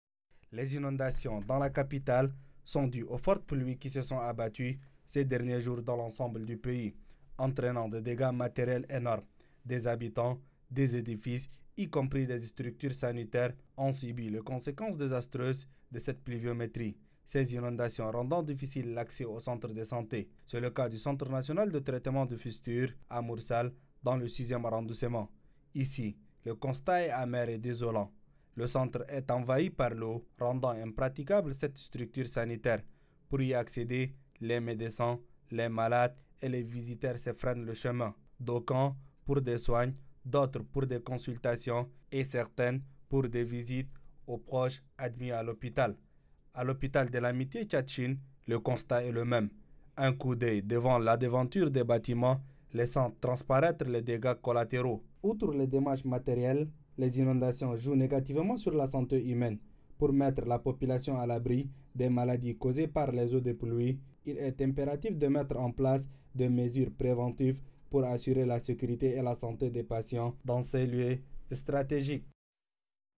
𝐈𝐍𝐎𝐍𝐃𝐀𝐓𝐈𝐎𝐍𝐒
𝐍'𝐃𝐣𝐚𝐦𝐞𝐧𝐚 𝐜𝐨𝐧𝐧𝐚𝐢𝐭 𝐮𝐧𝐞 𝐬𝐚𝐢𝐬𝐨𝐧 𝐩𝐥𝐮𝐯𝐢𝐞𝐮𝐬𝐞 𝐞𝐱𝐜𝐞𝐩𝐭𝐢𝐨𝐧𝐧𝐞𝐥𝐥𝐞. 𝐋𝐚 𝐯𝐢𝐥𝐥𝐞 𝐟𝐚𝐢𝐭 𝐟𝐚𝐜𝐞 𝐚𝐮𝐱 𝐢𝐧𝐨𝐧𝐝𝐚𝐭𝐢𝐨𝐧𝐬. 𝐋'𝐮𝐧𝐞 𝐝𝐞𝐬 𝐜𝐨𝐧𝐬𝐞́𝐪𝐮𝐞𝐧𝐜𝐞𝐬 𝐝𝐞 𝐜𝐞𝐭𝐭𝐞 𝐟𝐨𝐫𝐭𝐞 𝐩𝐥𝐮𝐯𝐢𝐨𝐦𝐞́𝐭𝐫𝐢𝐞 𝐡𝐨𝐫𝐦𝐢𝐬 𝐥𝐞𝐬 𝐢𝐧𝐨𝐧𝐝𝐚𝐭𝐢𝐨𝐧𝐬 𝐞𝐬𝐭 𝐥𝐞 𝐭𝐚𝐮𝐱 𝐞́𝐥𝐞𝐯𝐞́ 𝐝𝐞 𝐩𝐚𝐥𝐮𝐝𝐢𝐬𝐦𝐞 𝐞𝐧𝐫𝐞𝐠𝐢𝐬𝐭𝐫𝐞́ 𝐝𝐚𝐧𝐬 𝐥𝐞𝐬 𝐡𝐨̂𝐩𝐢𝐭𝐚𝐮𝐱. 𝐋𝐚 𝐬𝐢𝐭𝐮𝐚𝐭𝐢𝐨𝐧 𝐞𝐬𝐭 𝐩𝐫𝐞́𝐨𝐜𝐜𝐮𝐩𝐚𝐧𝐭𝐞; 𝐩𝐚𝐫𝐦𝐢 𝐥𝐞𝐬 𝐦𝐚𝐥𝐚𝐝𝐢𝐞, 𝐢𝐥 𝐧'𝐲 𝐚 𝐩𝐚𝐬 𝐪𝐮𝐞 𝐥𝐞 𝐩𝐚𝐥𝐮𝐝𝐢𝐬𝐦𝐞. 𝐀𝐥𝐡𝐚𝐛𝐢𝐛 𝐌𝐚𝐡𝐚𝐦𝐚𝐭 𝐍𝐨𝐮𝐫